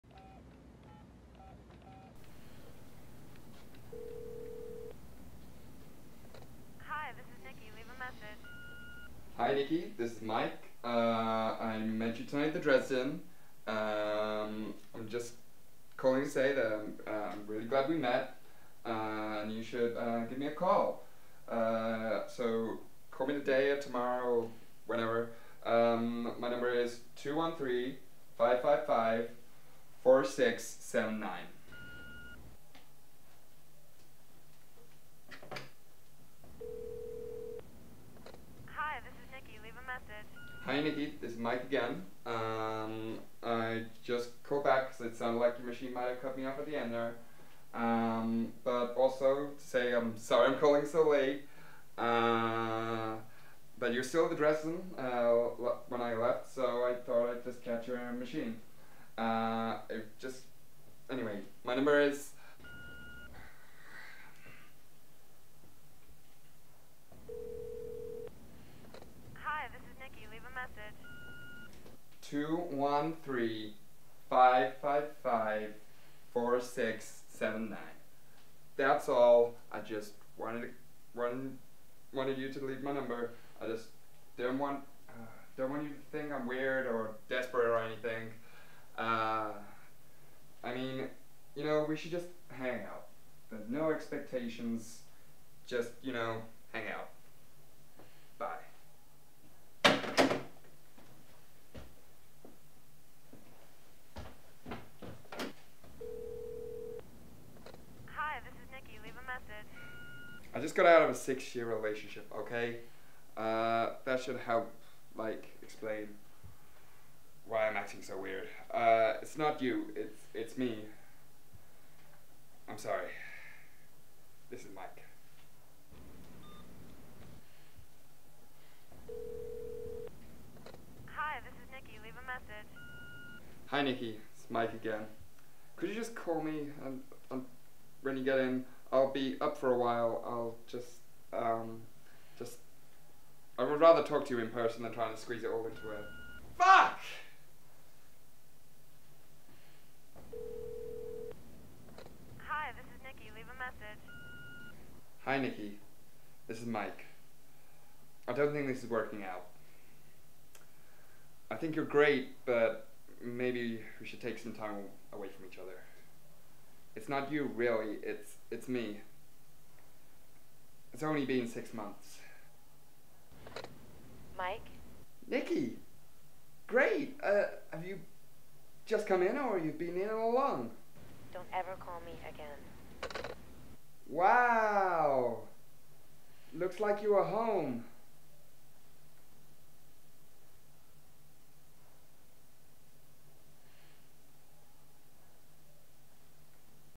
Swingers Voicemail
Second Year Scene Recreation for a unit